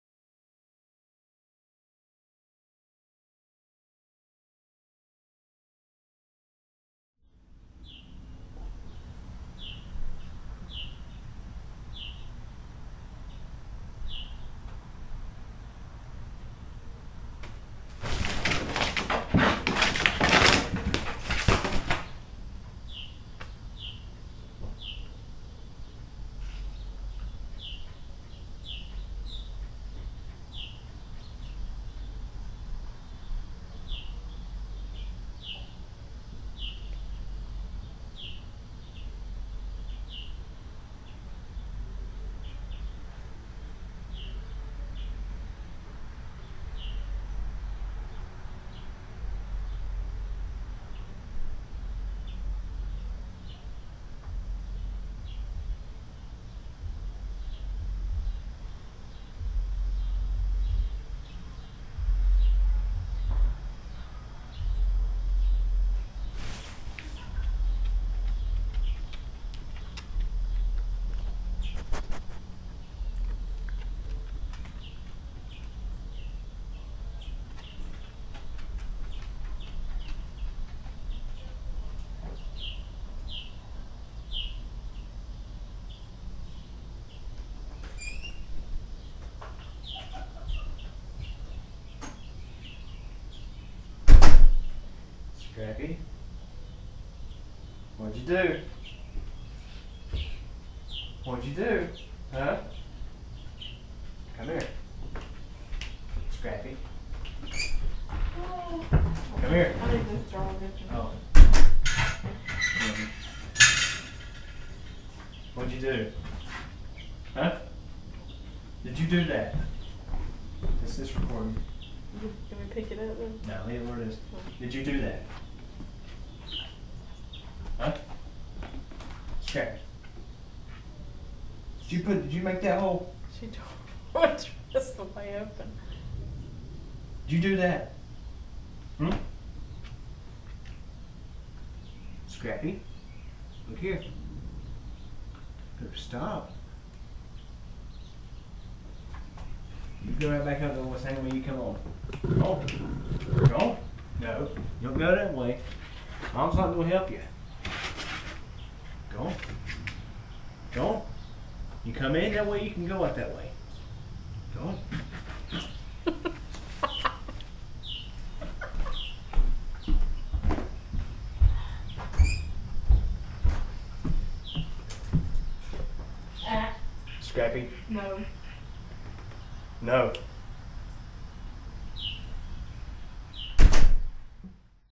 Dog got CAUGHT on camera! (Read Description)
Our dog Scrappy was getting on the porch even though all the screen doors were closed. There is a big sheet of plastic covering an open space where a window is about to get installed. Turns out, Scrappy is been punching through the plastic to get onto the porch! Watch as she fails at the first attempt. She makes it up on the second try, then goes and sniffs my camera. I then escorted her off the porch the same way she came in.